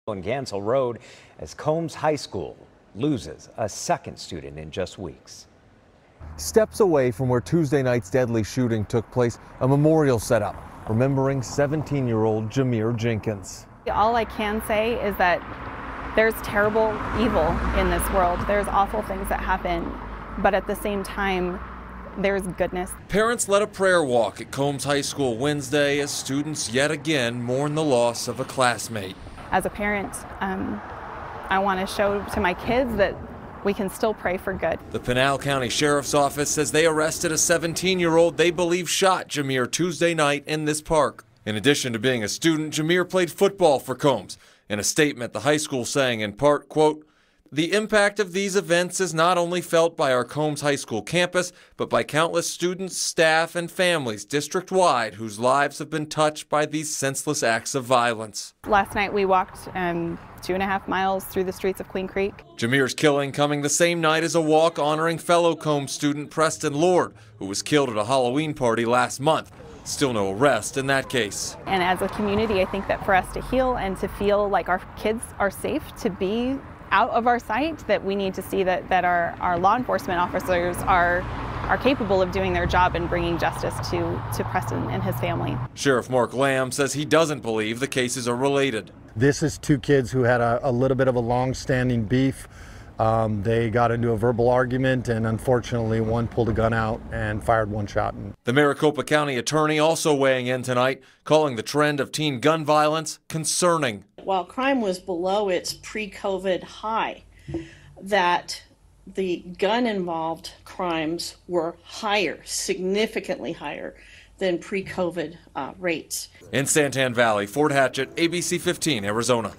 Teenager killed after argument in San Tan Valley
17-year-old Jamiere Jenkins was a student at Combs High School, the district has confirmed.